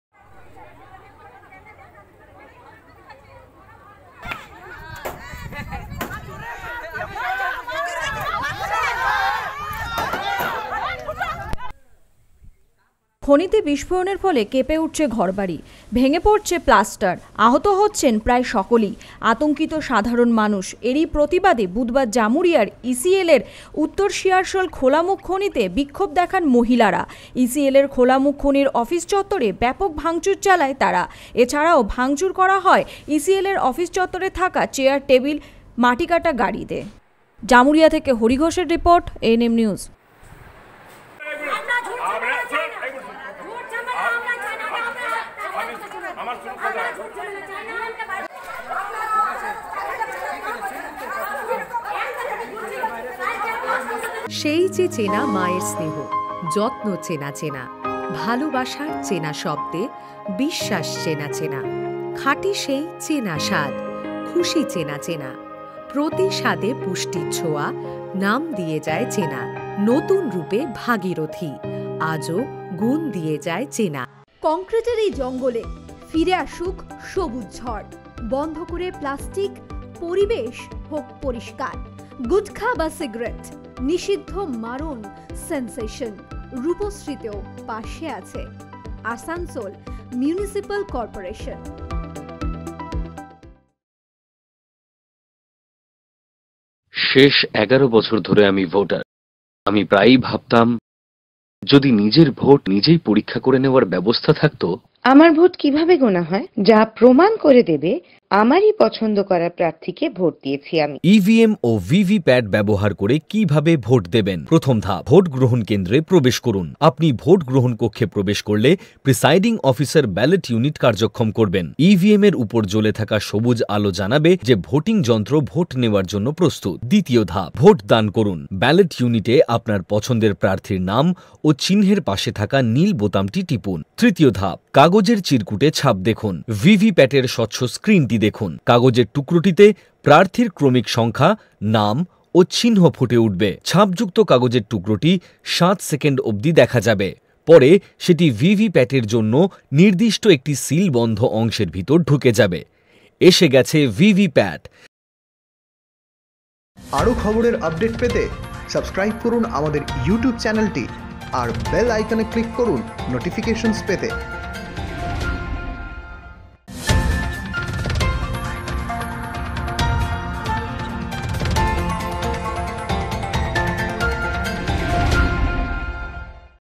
জামুরিয়ায় কয়লা খনিতে অবৈজ্ঞানিক বিস্ফোরনে ভাঙছে বাড়ি ,বিক্ষোভ মহিলাদের ||05152019||ANMNEWS
হরি ঘোষ ,জামুরিয়া :খনিতে বিস্ফোরণের ফলে কেঁপে উঠছে ঘরবাড়ি , ভেঙে পড়ছে প্লাস্টার ,আহত হচ্ছেন  প্রায় সকলেই ,আতঙ্কিত  সাধারণ মানুষ।এরই প্রতিবাদে বুধবার জামুড়িয়ার ইসিএলের উত্তর সিয়ারসোল খোলামুখ খনিতে বিক্ষোভ দেখান মহিলারা।ইসিএলের খোলামুখ খনির অফিস চত্বরে ব্যাপক ভাঙচুর চালায় তারা । এছাড়াও ভাঙচুর করা হয় ইসিএলের অফিস চত্বরে থাকা চেয়ার, টেবিল,মাটিকাটা গাড়ি।